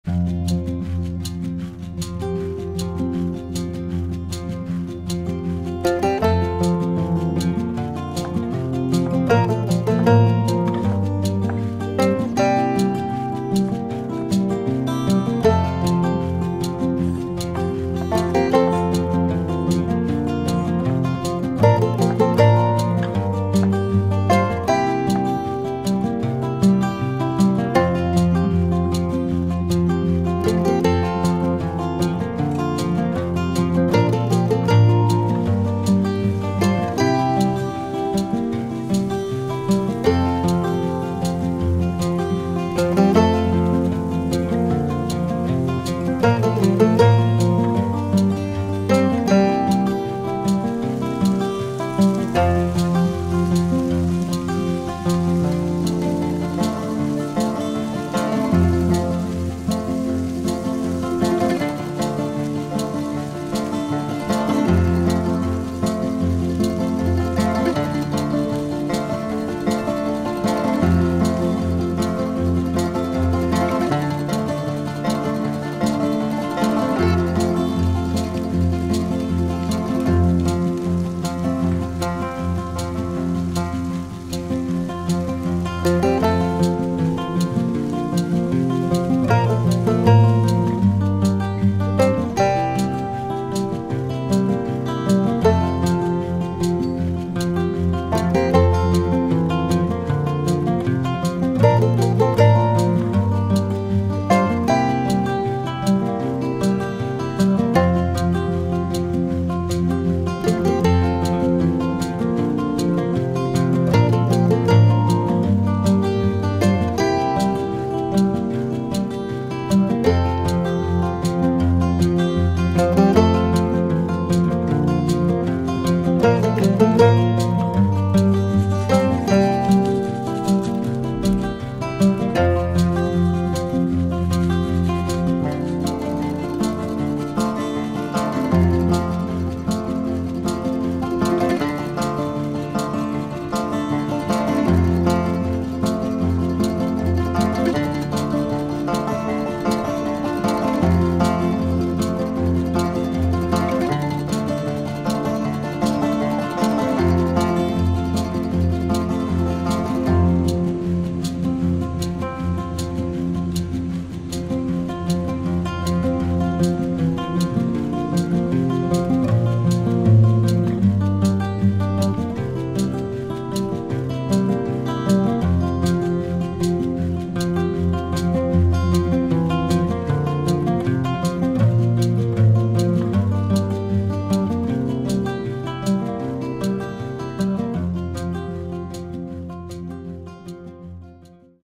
Baked beans Bruschetta like a boss
Super easy recipe with beans, tomatoes and fresh bread. So delicious, high in protein and ready in 5 minutes.
You only need canned beans, cherry tomatoes, fresh bread, one onion, olive oil, salt, pepper, garlic and fresh parsley.